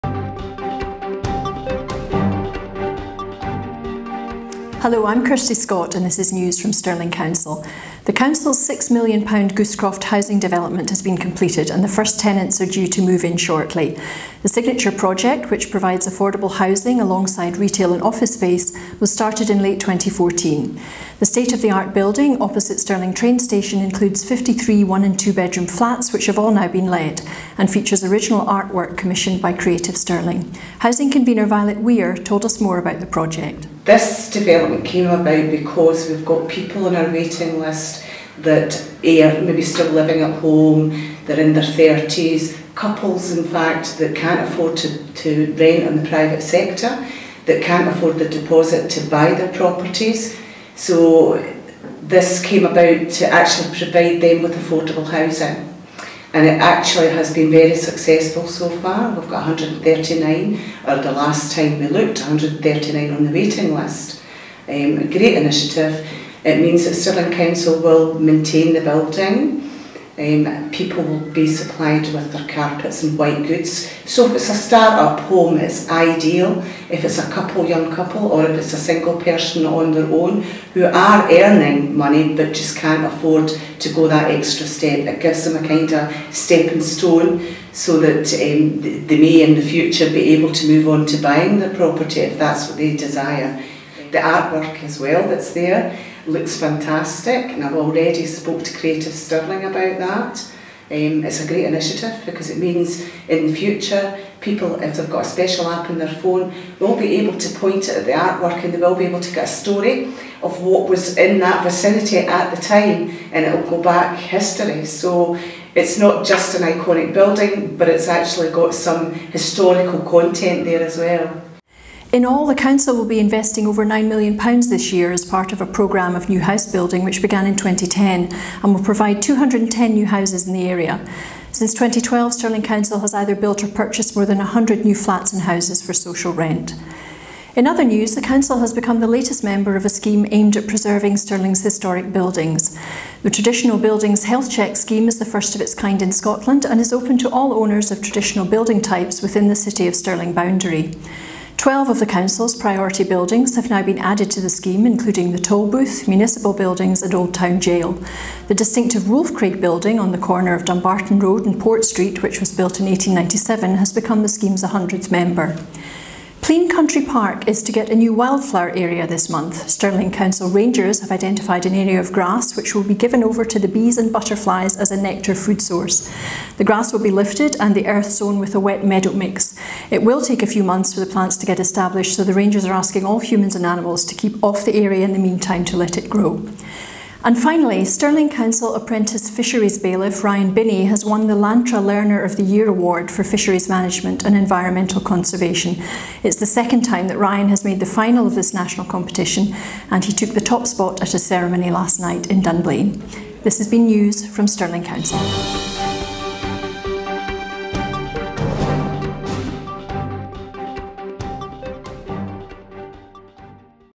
News Bulletin 4th March
Stirling Council is launching a new digital Council news bulletin to provide information on what’s happening across the Council and the Stirling area. We’d love to know what you think.